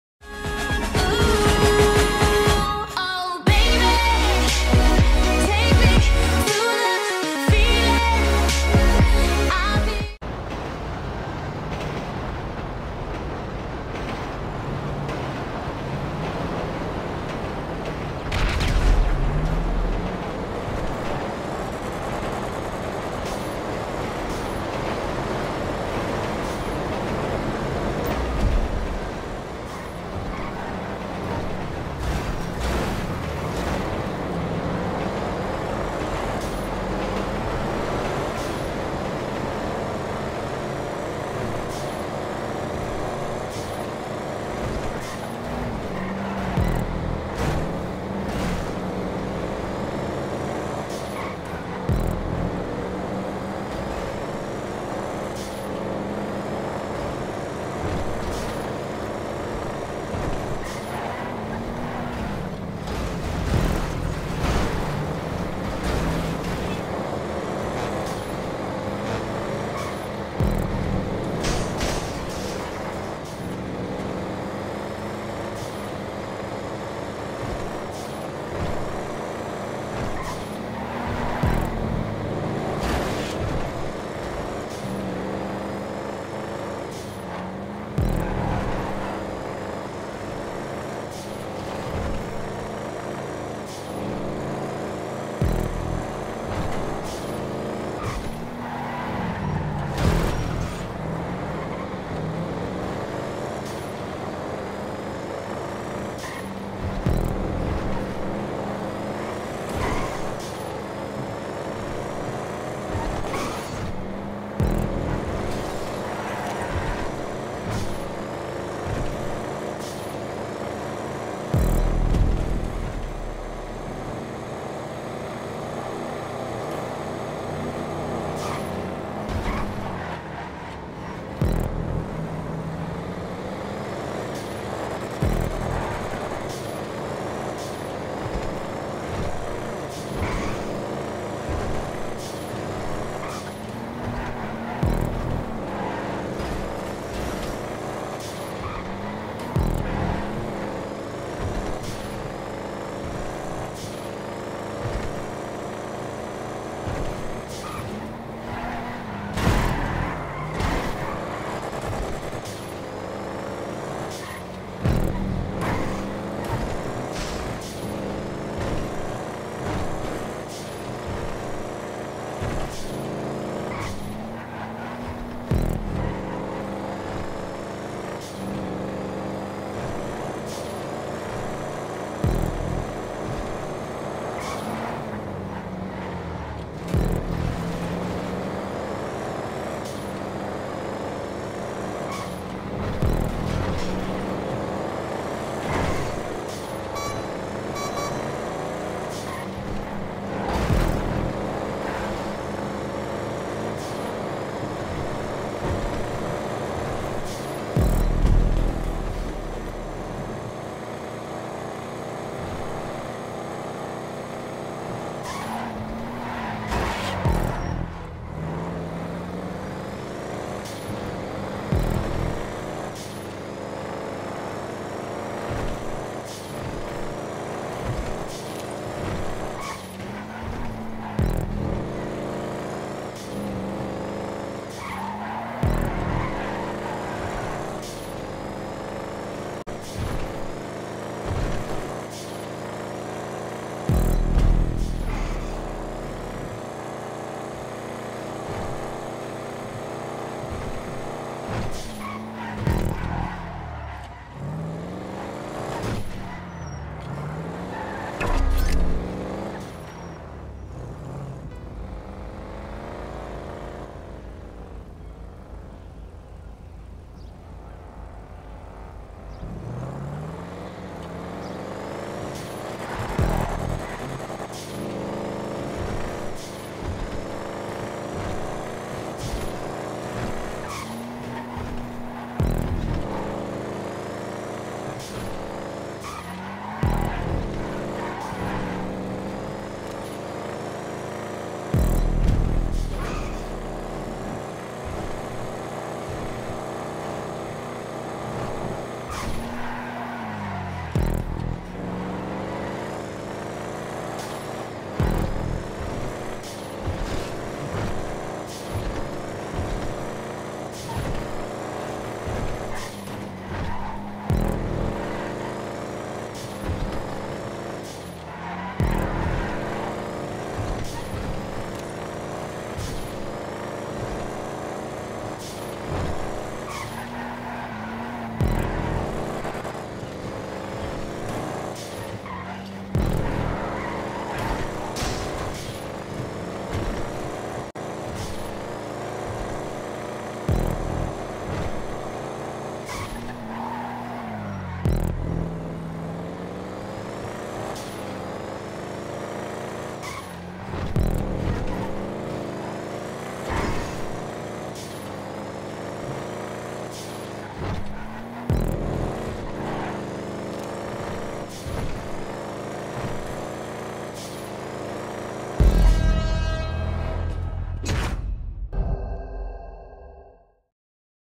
COTM | S2 | R2 | D2 (Highlights)
Thought I did pretty well considering I started near the back and hardly even practiced. Plus apparently there is some secret setup for this car... Oh well, still very happy with the outcome was a very fun race! Keen for next week :)

That pit exit though ;)